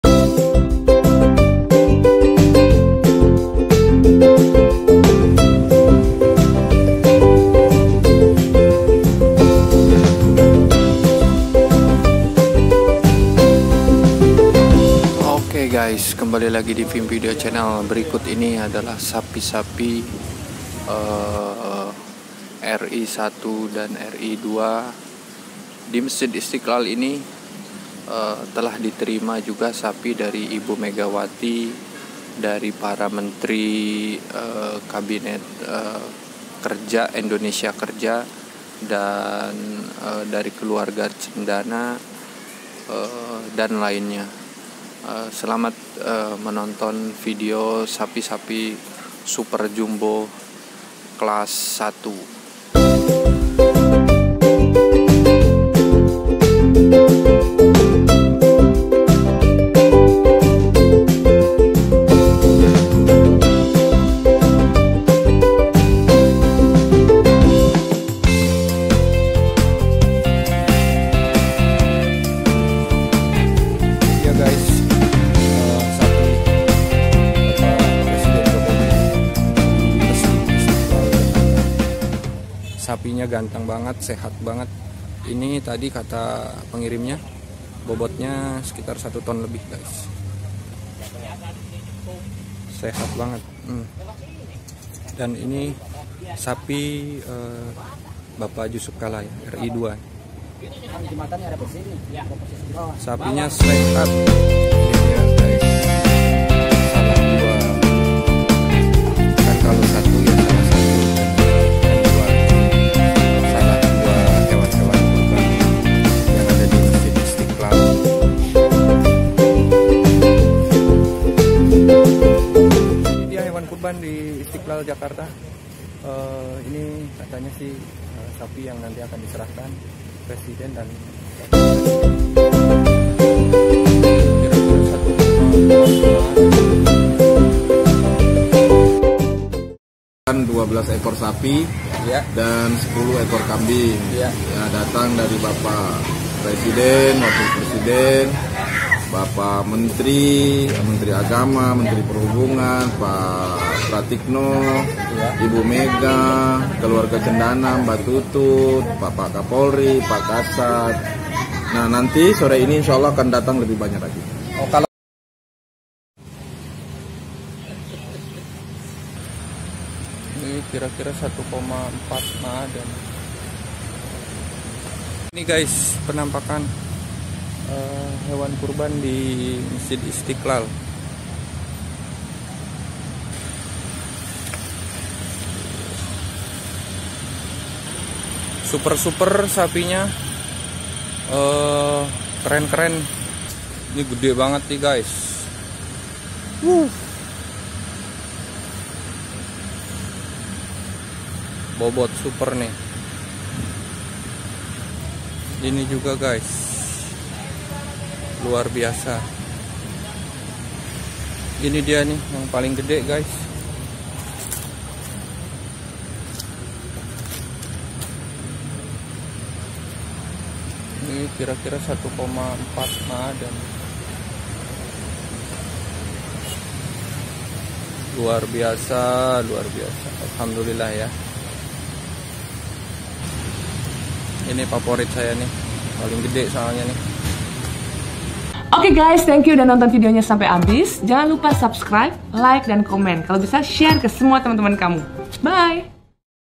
Sapi Qurban Masjid Istiqlal - Sapi Sapi Istimewa
Persiapan Menyambut Hari Raya Idul Adha Masih Istiqlal Telah menerima sapi dari Presiden dan Wakil Presiden RI. tidak hanya itu, Panitia Qurban Masjid Istiqlal Ustadz Abu Hurairah mengungkapkan telah menerima Sapi Qurban Dari Ibu Megawati dan Para Menteri juga Keluarga Cendana.